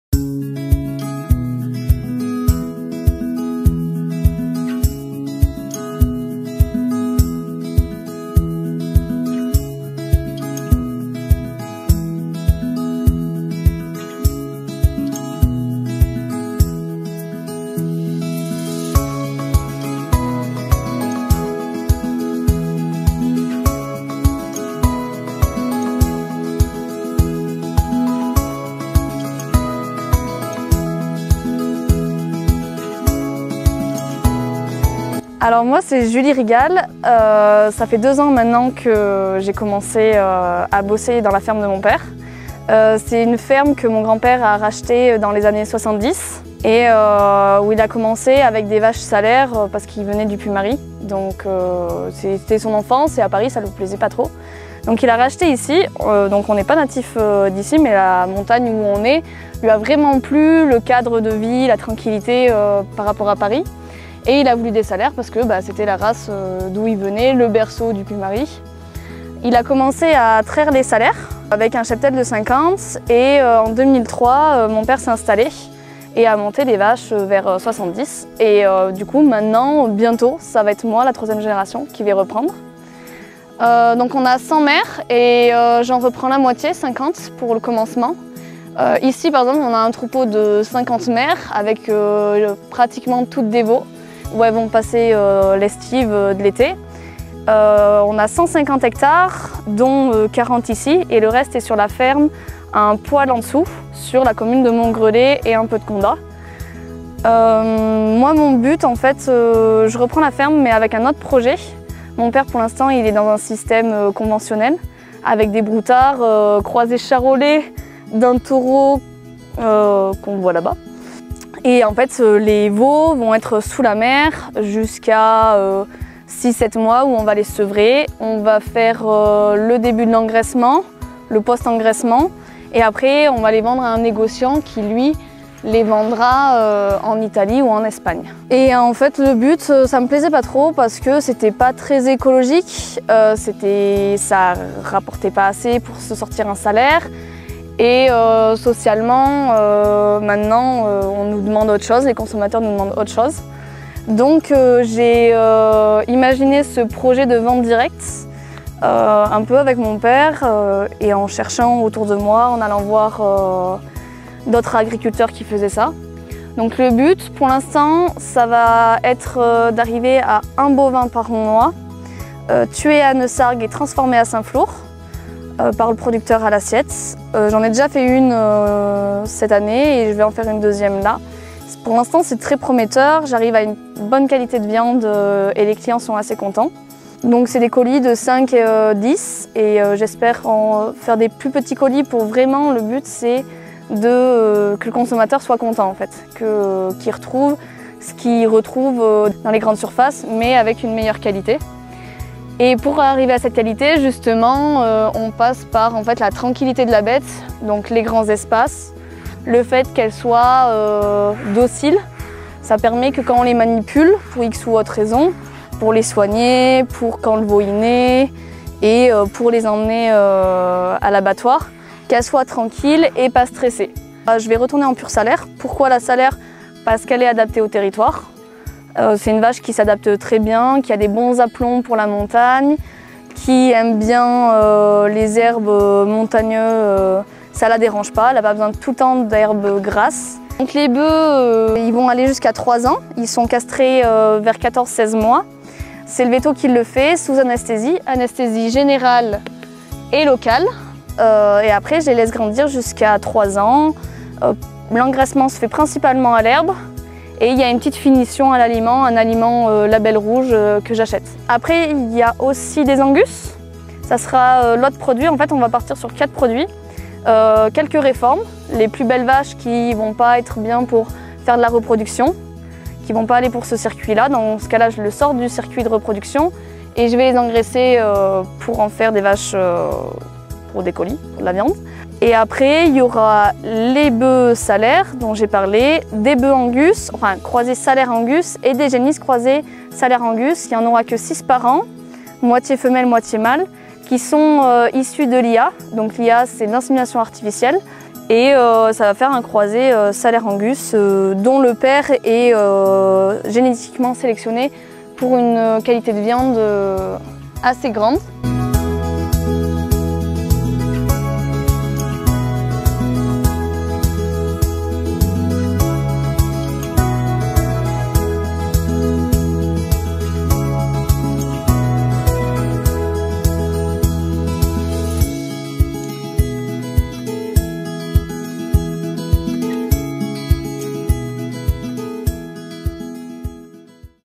Un élevage de Salers dans les montagnes de Montgreleix
Découvrez l'histoire, le troupeau et notre engagement pour notre exploitation de demain
Vidéo réalisée en août 2023, première interview pour mes vaches et moi.
Très fière de celles ci qui se sont montrées calme et intéressées malgré la chaleur.
Si vous voulez en savoir plus le lien du site ci-dessous

Et pour voir d'autres vidéos avec des images comme celle ci
Instagram/ Facebook/ Youtube : Antom Production